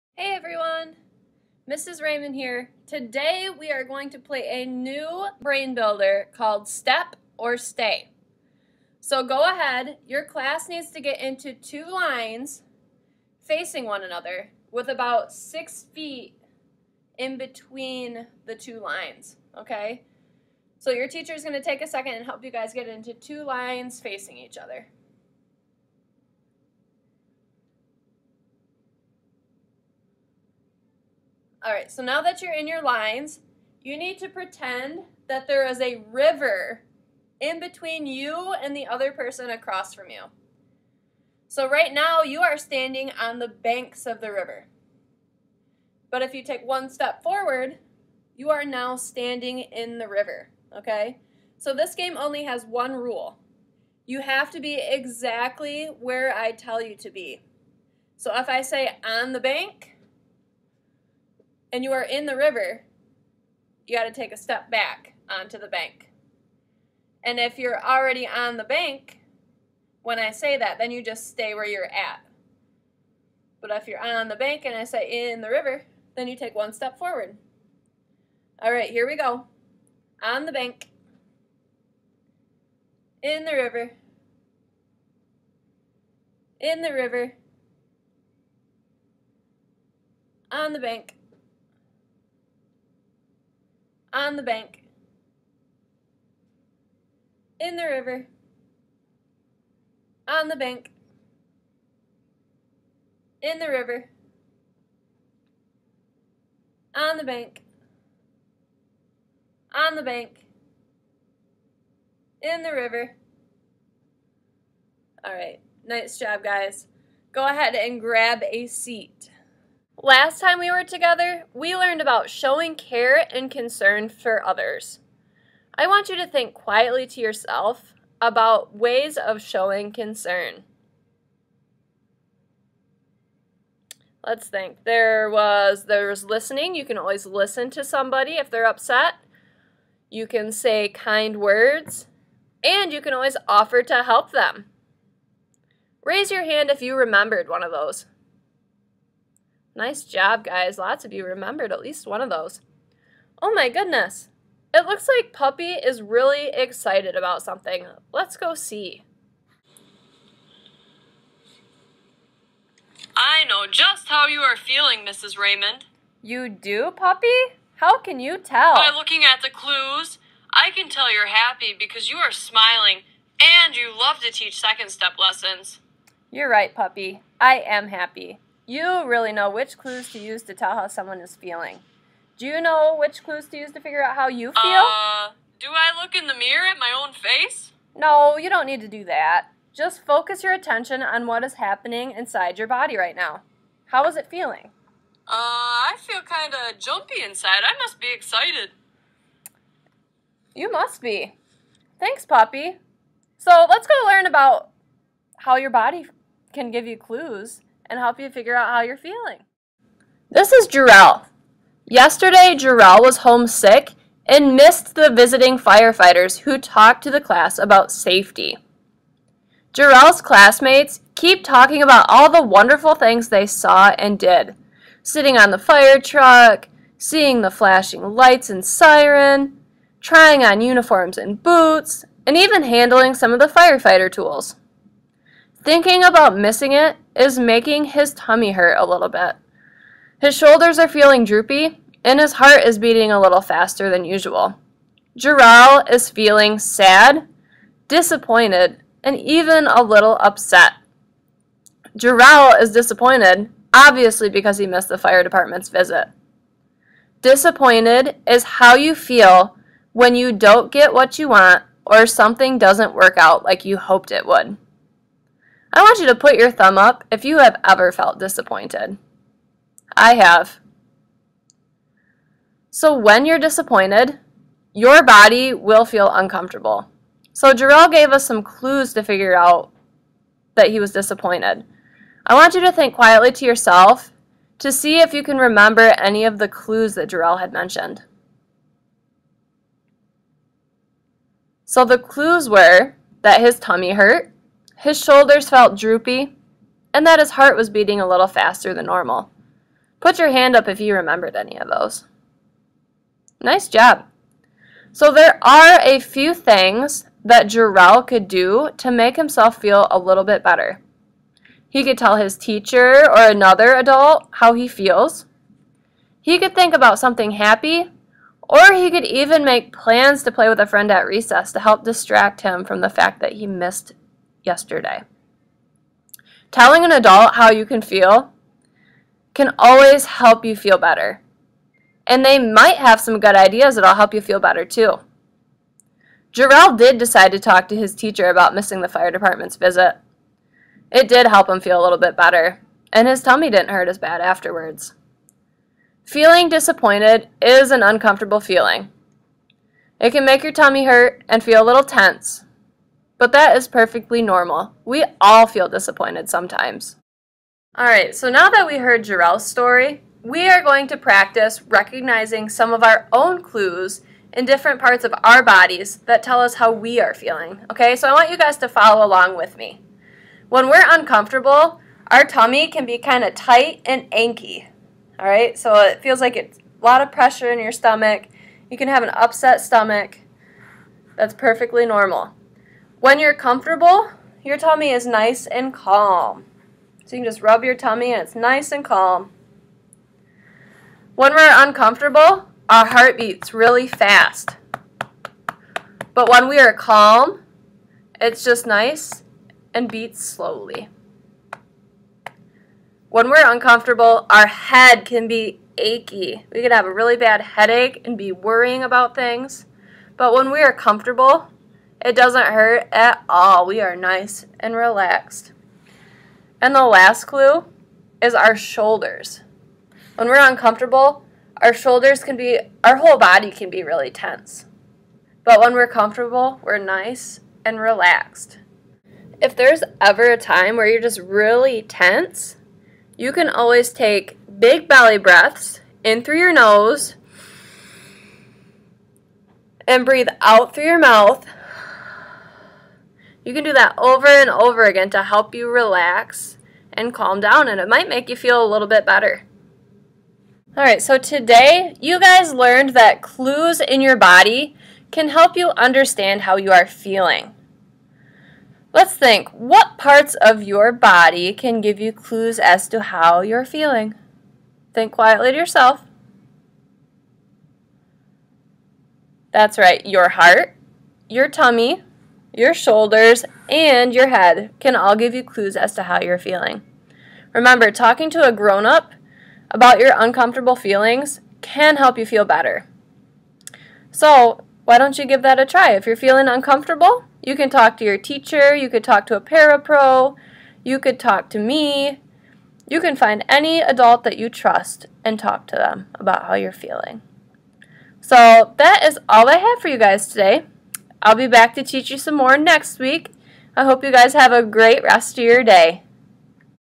Second Step 1st Grade Lesson 12: Identifying Our Own Feelings
This video was created for Central Montcalm Elementary. The purpose of this video is to provide Second Step curriculum in a virtual format